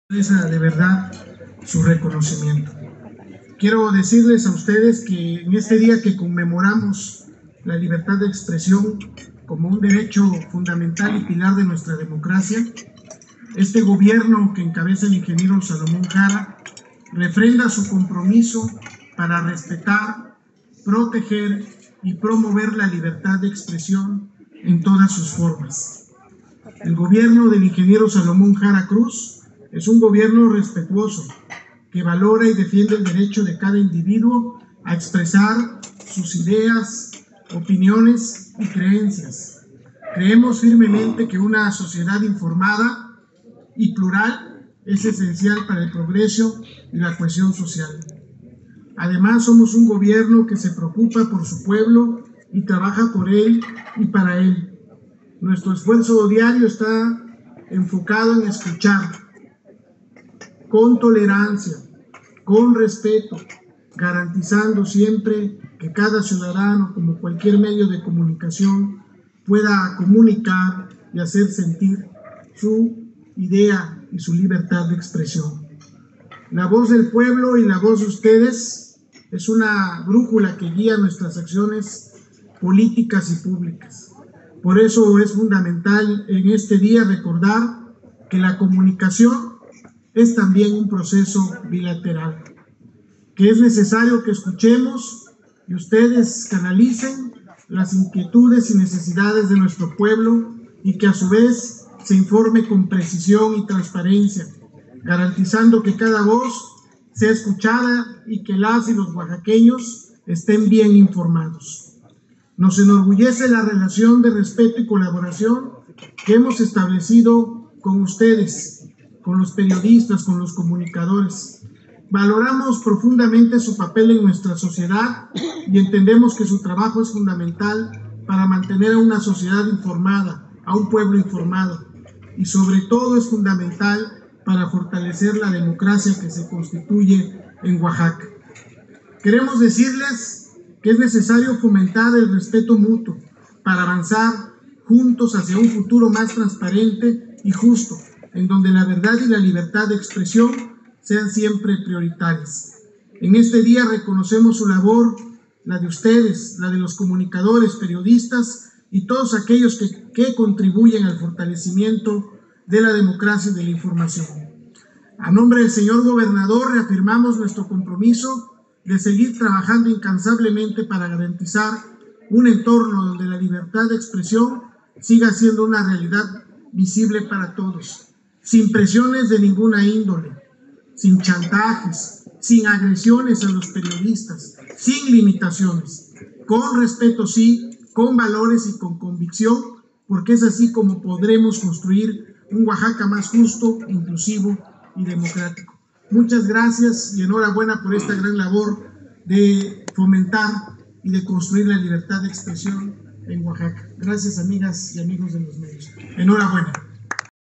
Reafirma Gobierno de Oaxaca compromiso con la libertad de expresión: Jesús Romero
“El Gobierno del Estado de Oaxaca reafirma su compromiso para respetar, proteger y promover la libertad de expresión en todas sus formas”, señaló el titular de la Secretaría de Gobierno (Sego), Jesús Romero López, en representación del Gobernador Salomón Jara Cruz, durante un encuentro alusivo al Día de la Libertad de Expresión.